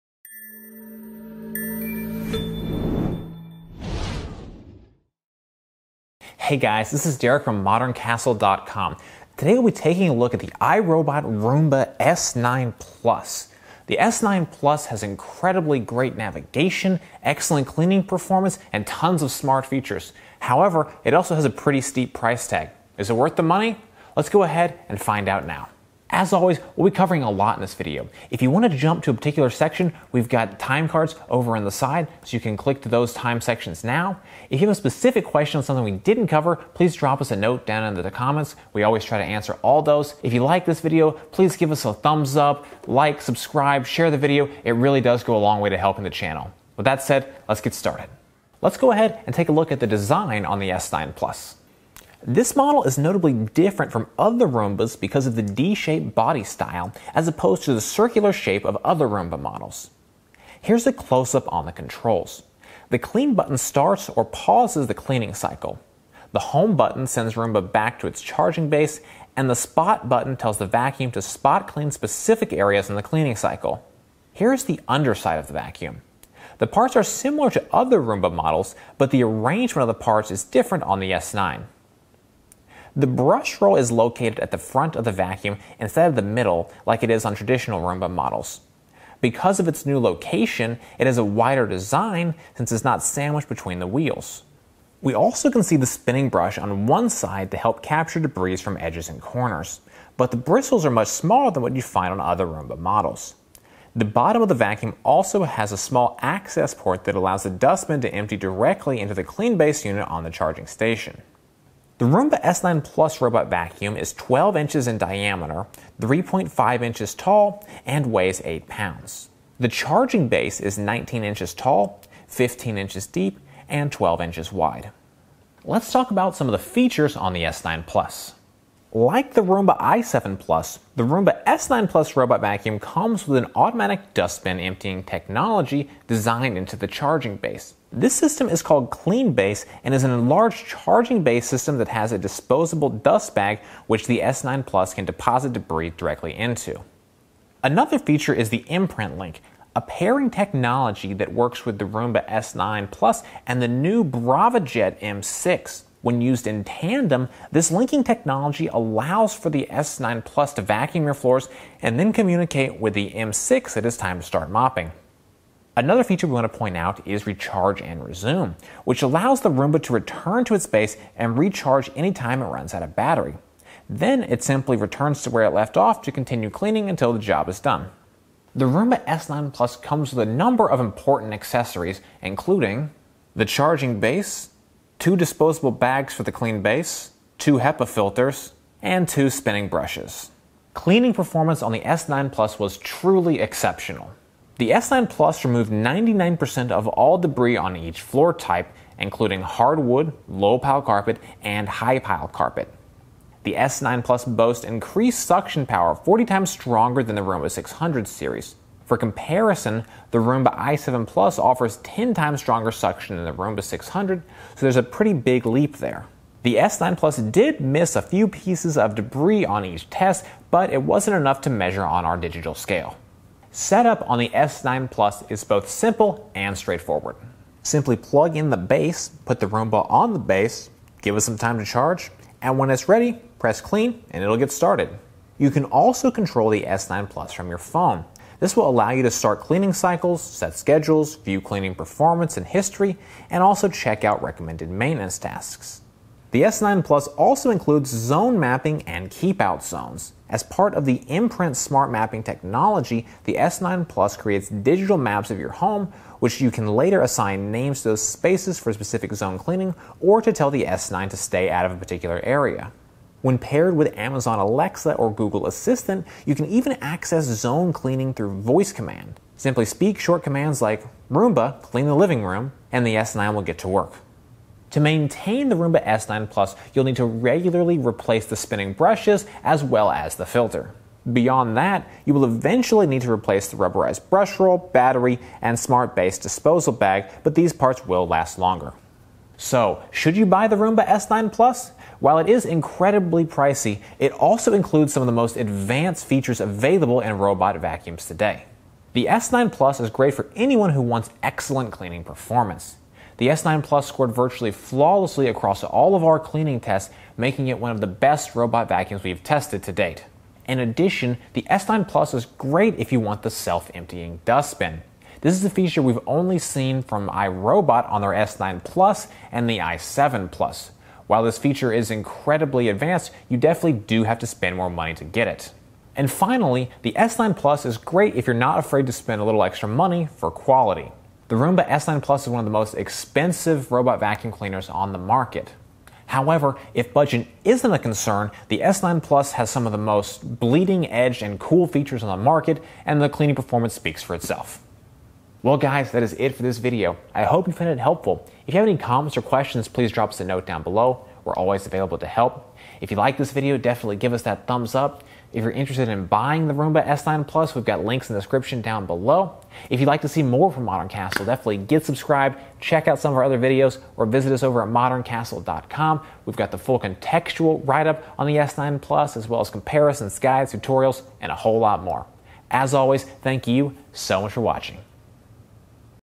Roomba S9+ Review
Roomba S9+ is the newest robot from iRobot, and also the most advanced robot they make. Is it worth the money? Find out in our Roomba S9+ review now.

0:00 Intro
0:51 Design
2:09 Features & Accessories
3:13 Cleaning Tests
3:46 Setup & Maintenance
4:52 Summary

For the best-of-the-best that Roomba has to offer, the Roomba S9+ robot vacuum is an amazing machine. While it is incredibly pricey, it also comes with the most advanced features on the robot vacuum market.

I would recommend the Roomba S9+ if you want the following in a robot vacuum:

Want a self-emptying dustbin: Aside from the Roomba i7+, the Roomba S9+ is one of the only other models on the market to offer a self-emptying dustbin. This feature can be useful if you want a “set it and forget it” approach to your robot vacuum.

Want a square front body: For the first time, iRobot has designed a square front robot vacuum with the Roomba S9+. While the body style certainly does remind us of comparable models to come from Neato Robotics, it’s a big change for Roomba. In addition to excellent edge cleaning, the square front body also allows for a wider cleaning path with longer brushrolls.

Want excellent cleaning performance: It scored 99% on all 3 of our floor cleaning tests. It offers incredible suction and that suction results in incredible cleaning performance. The Roomba S9+ is among the very best robot vacuums we have tested to date.

We are always happy to help!